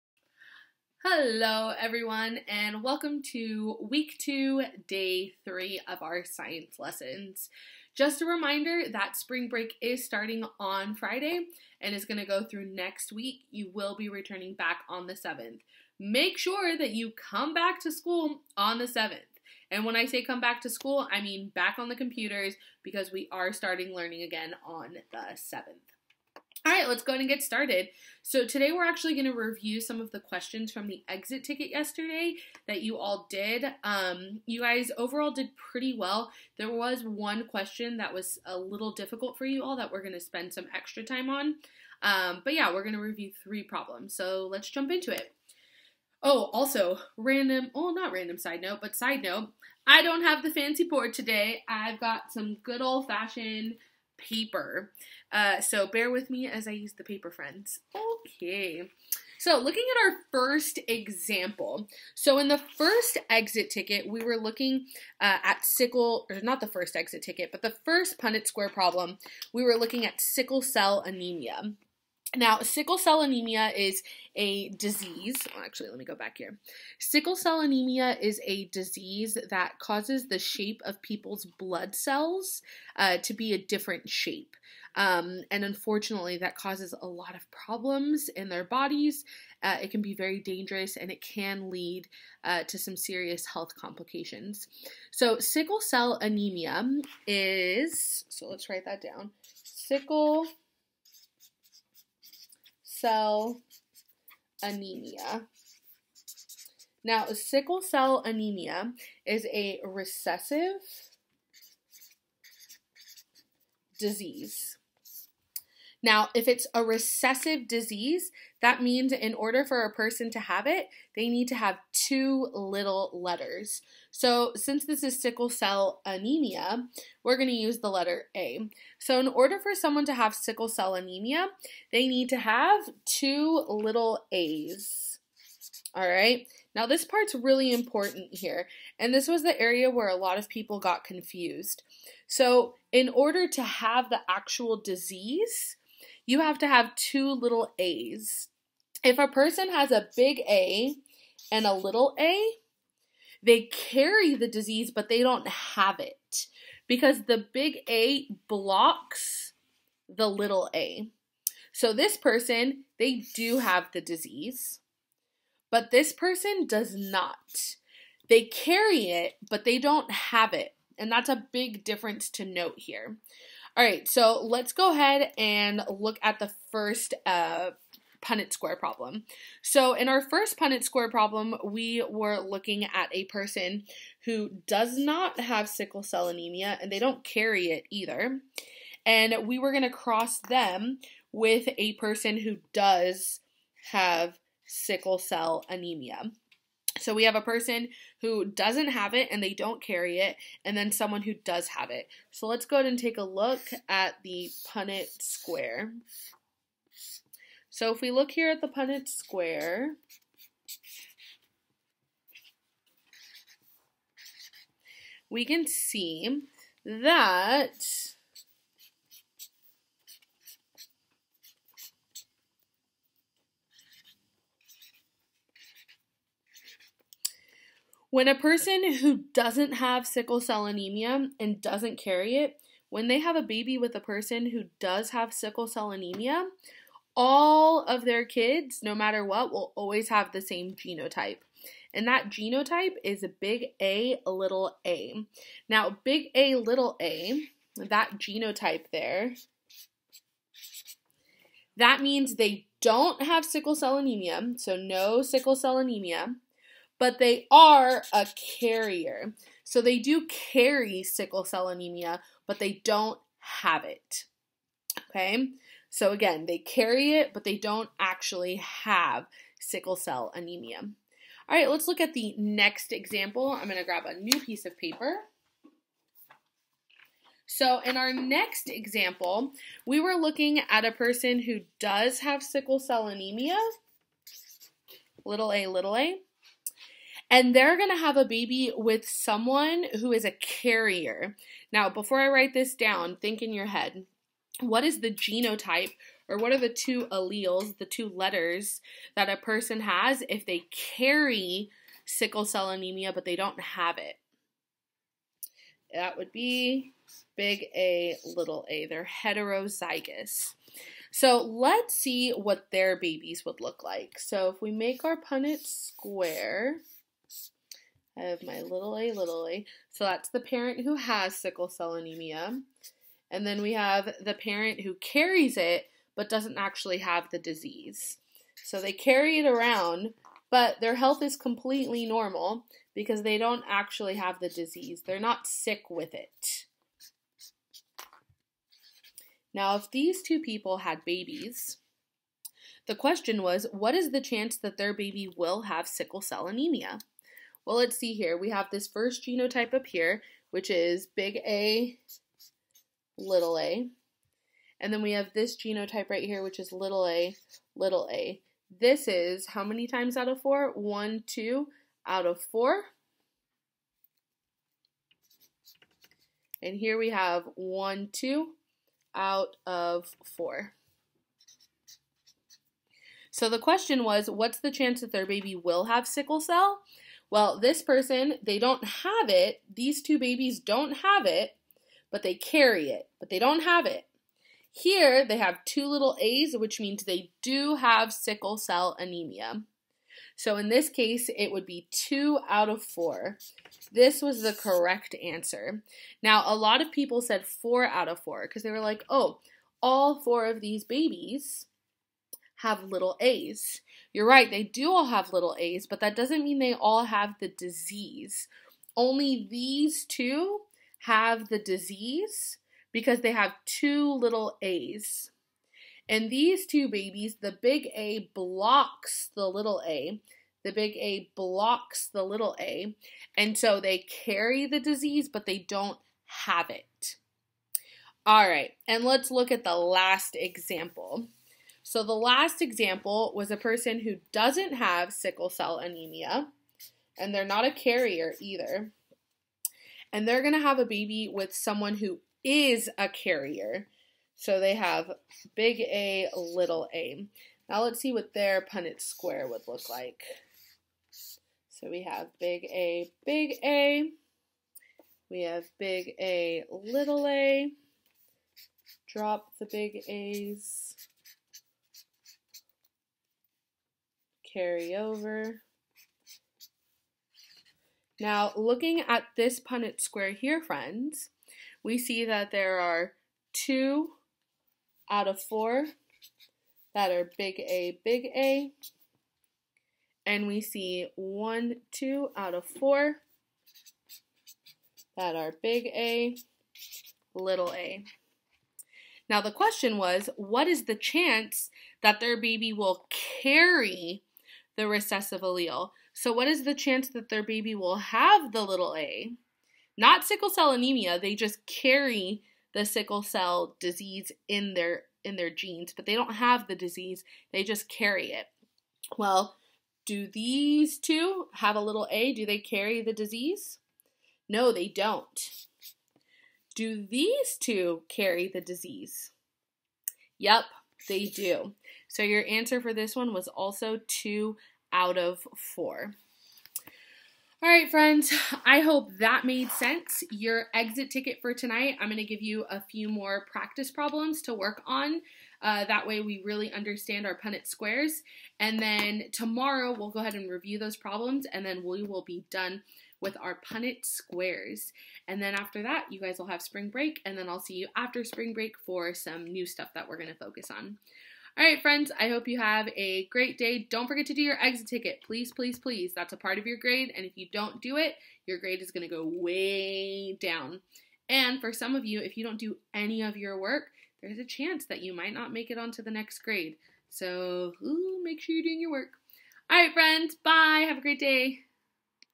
Lesson 8
This lesson reviews several questions from our Exit Ticket.
Don't forget, in a recessive disease or disorder, a carrier only carries the disease. It doesn't mean they have the disease.

HOMEWORK
Complete the exit ticket on Google Classroom.